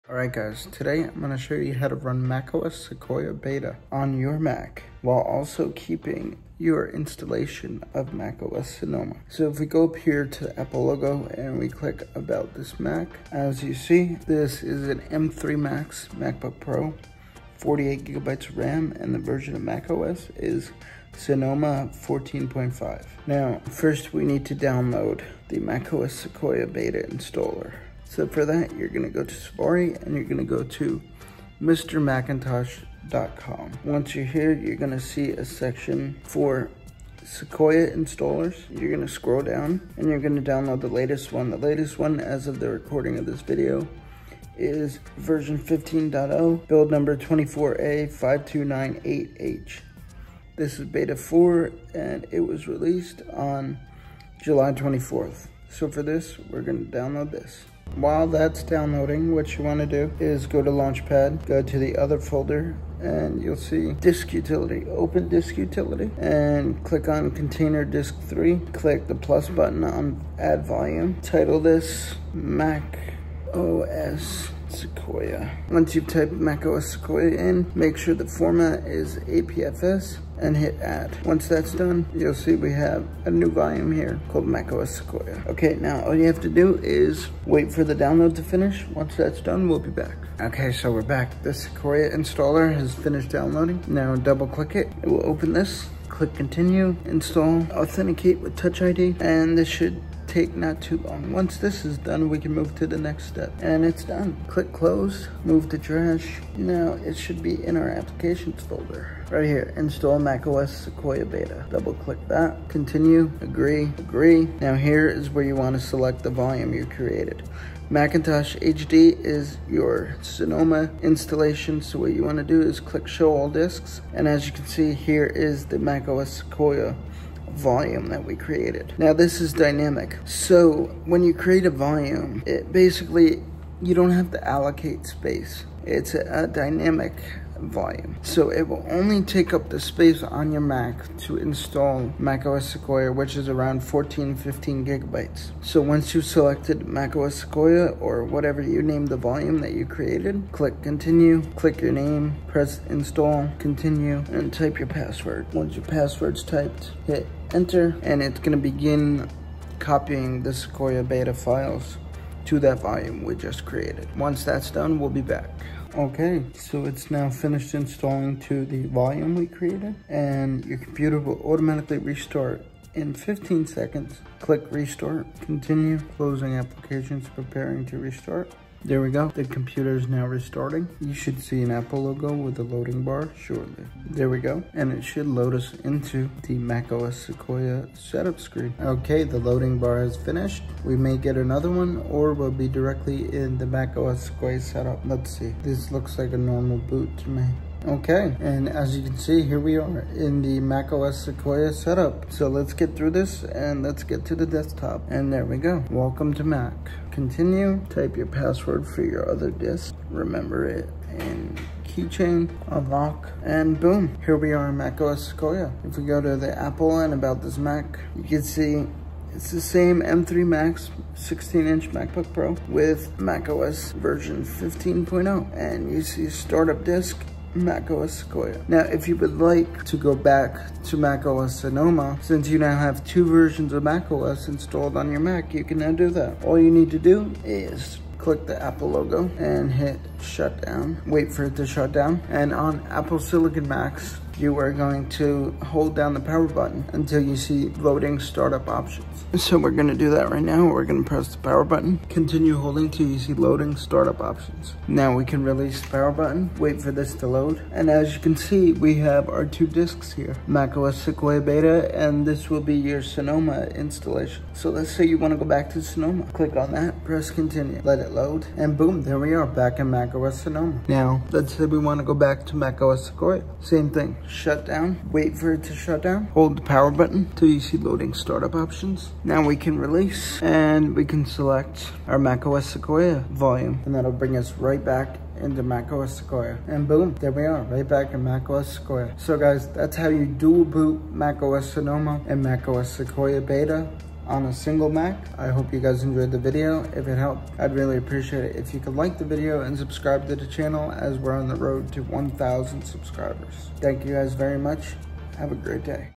How to Dual Boot macOS Sequoia beta and macOS Sonoma on your Mac!
In this video, we go over how to set up two different macOS installations on the same Mac. in this case it was macOS Sequoia beta, and macOS Sonoma.